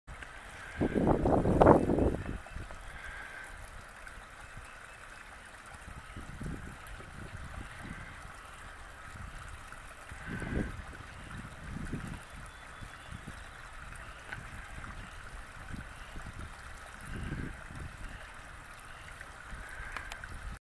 Marly Artesian Spring, Pearl's Fen, Greene County, Ohio
Cold, alkaline groundwater welling up through marl deposits.